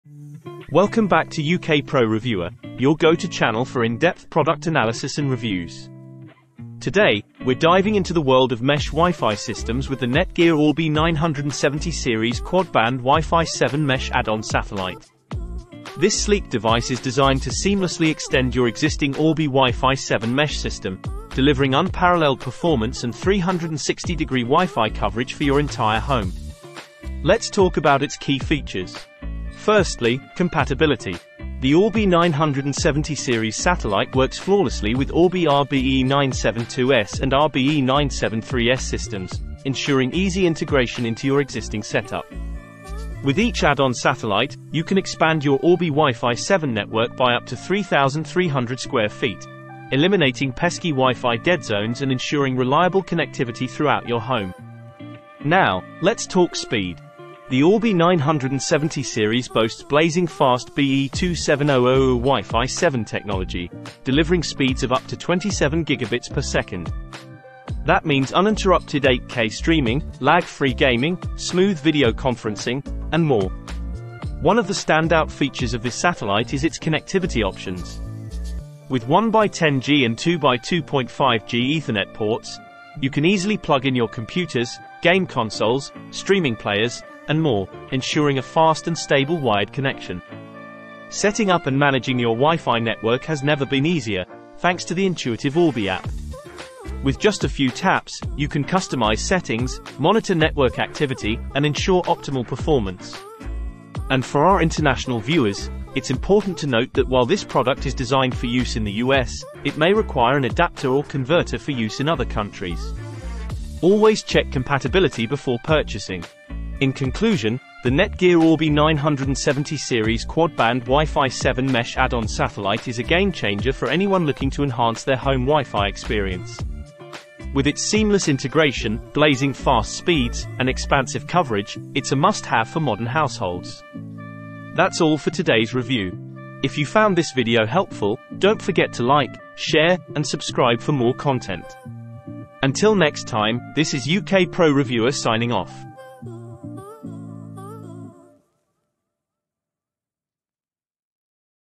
📡 NETGEAR Orbi 970 Series Quad-Band WiFi 7 Mesh Add-on Satellite (RBE970) Review 📡
Amazon Product Name : NETGEAR Orbi 970 Series Quad-Band WiFi 7 Mesh Add-on Satellite (RBE970)

Discover unmatched Wi-Fi performance with the NETGEAR Orbi 970 Series Quad-Band WiFi 7 Mesh Add-on Satellite (RBE970) in this comprehensive review. Delve into the features and capabilities of this innovative mesh satellite, meticulously engineered to extend the coverage and speed of your Orbi WiFi system. From its quad-band technology to its seamless integration with existing Orbi setups, explore how the NETGEAR Orbi 970 Series Satellite enhances your home network. Whether you're streaming, gaming, or working from home, this review covers everything you need to know about the NETGEAR Orbi 970 Series Quad-Band WiFi 7 Mesh Add-on Satellite (RBE970). Join us as we delve into the functionalities and benefits of this essential networking solution.